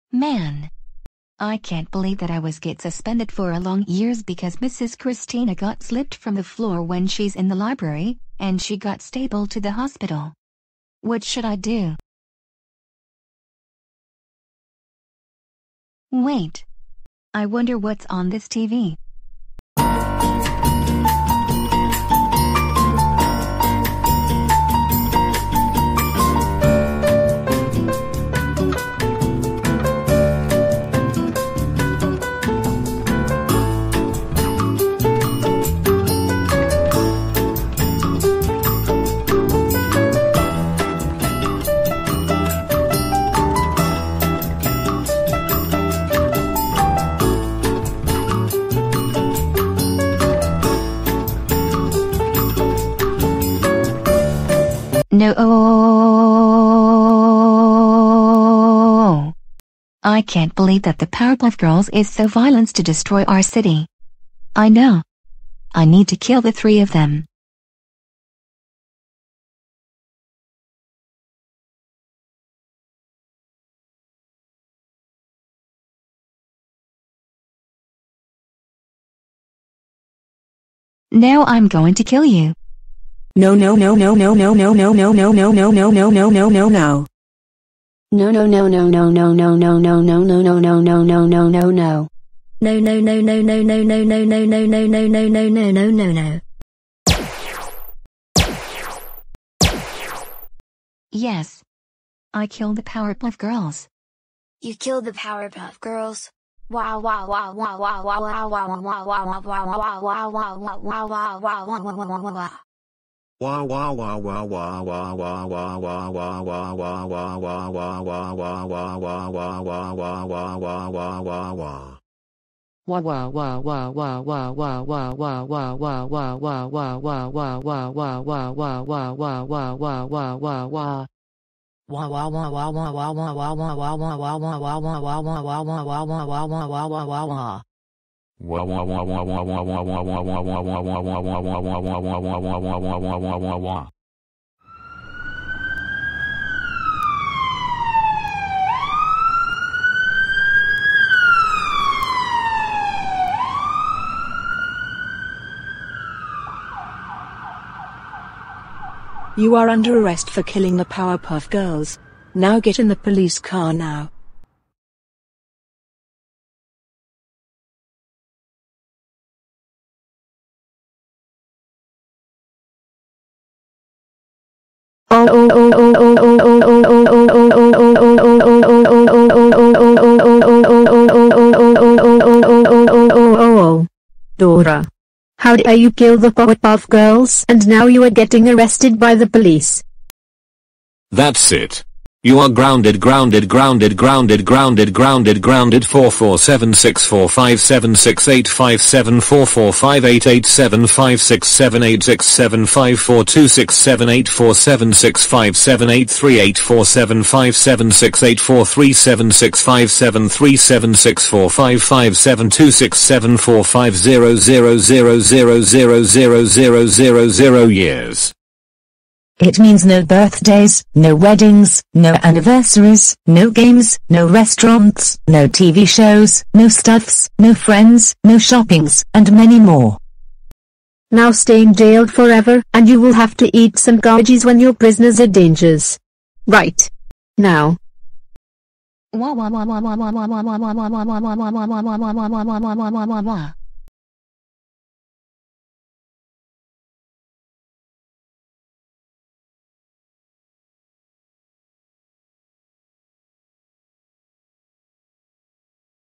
Dora Kills the Powerpuff Girls/Arrested/Grounded
PLOT:
When Dora was bored in the living room after she gets suspended for the long days because she putting some oils on the floor when Mrs. Christina slipping from the library, she thinks that she should watch TV she grounds by her mother, but anything else. she put the remote and she watched.
Then, the TV aired the Powerpuff girls casts: Blossom, Bubbles, and Buttercup are regular from the shows, which is the scene fights against the GoVille city for the justice heroes, and the children are cheered for arriving the Powerpuff girls lovely!
But, Dora gets angered for hating Powerpuff girls on the TV, even she will get her gun to kill them.
When she walked through on the screen TV, the Powerpuff Girls gets killed because she hates that, but the children gets shocked, and they're crying!
Even, Dora gets arrested by the fat policewoman for killing the Powerpuff Girls, and the TV was interrupted.
Long minutes later before Dora was in jail, she gets grounded by her parents, because she deserves for being a troublemaker, and she will jailed forever to eat some garbage from the prisoners, and then she cried forever while her parents are walked out.
THE END!!!

Cast:
Dora: Julie
Veena: Herself
Miguel: Daniel
Abuela: Catherine
Blossom: Allison
Bubbles: Salli
Buttercup: Emma
Boy #1: Justin
Boy #2: Brian
Girl #1: Ashley
Girl #2: Kendra
Boy #3: Joey
Fat Policewoman: Amy